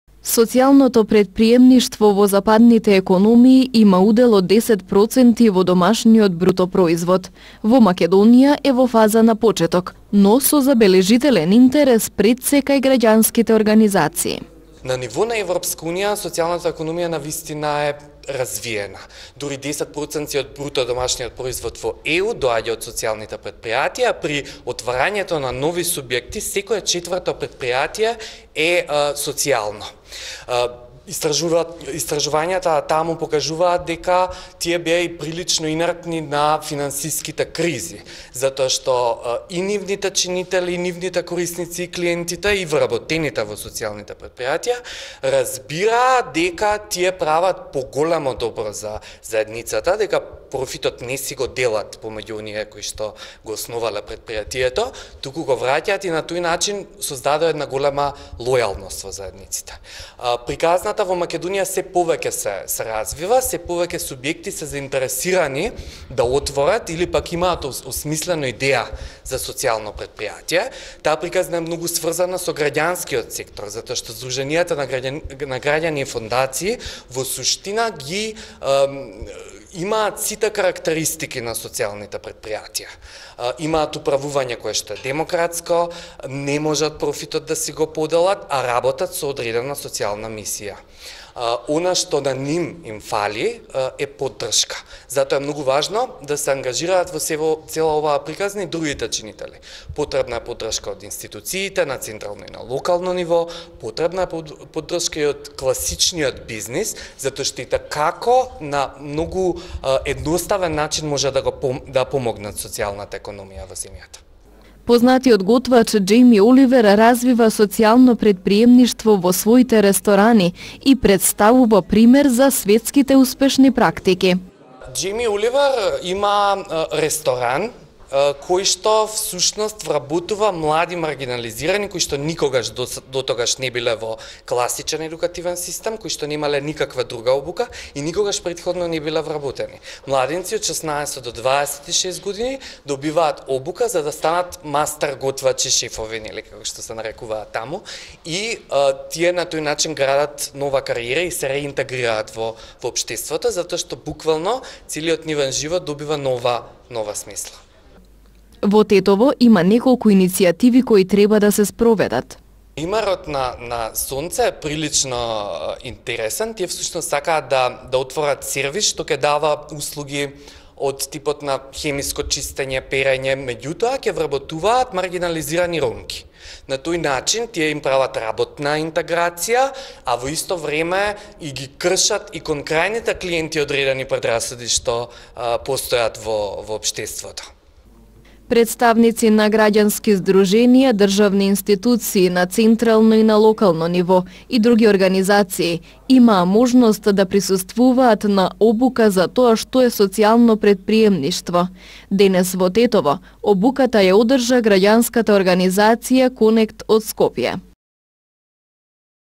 Социјалните претпријатија со многу придобивки
Вработувања и надминувања на предрасуди е придобивката од социјалните претпримеништва или претријатија. Во светот финансикиот обрт е значаен дел од домашната економија.